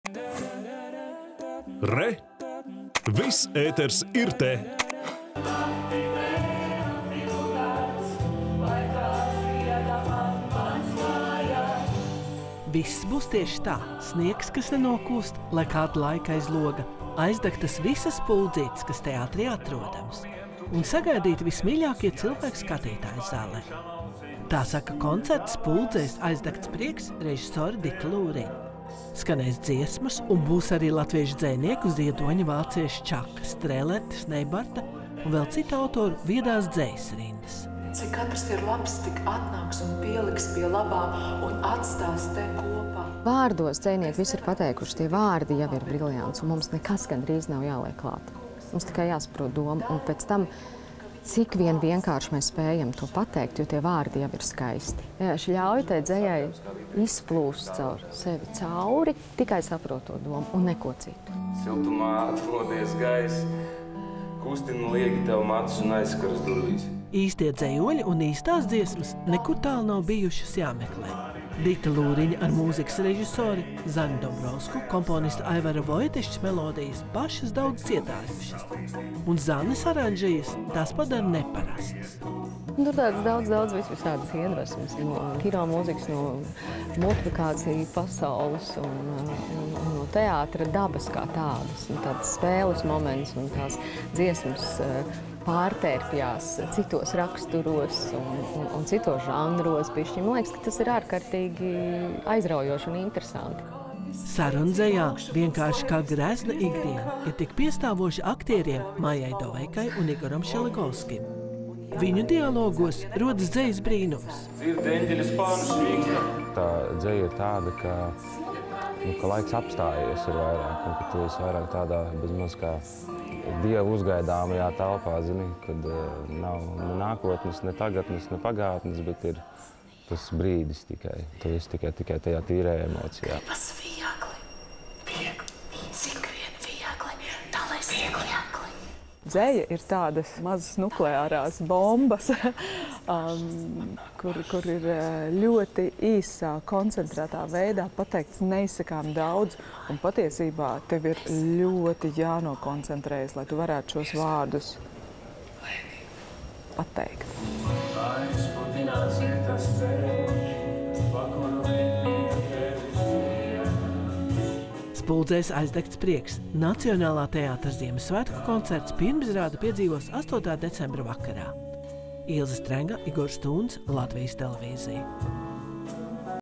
Spuldzēs aizdegts prieks. Nacionālais teātris Ziemassvētkos
“Spuldzēs aizdegts prieks” – tik dzejiski savu Ziemassvētku laika programmu piesaka Latvijas Nacionālais teātris.
  Poētisko rindu reiz rakstījusi dzejniece Biruta Skujeniece un tieši latviešu dzejnieku dzejai svētku koncertā būs nozīmīga loma.
   Dzeju un dziesmas koncertam izvēlējās režisore un aktrise Dita Lūriņa kopā ar mūzikas aranžētāju Zani Dombrovsku.
   Šovakar , 8.decembrī  “Spuldzēs aizdegts prieks” iemirdzēs pirmizrādē – uz Latvijas Nacionālā teātra Lielās zāles skatuves. Stāsta žurnāliste Ilze Strenga.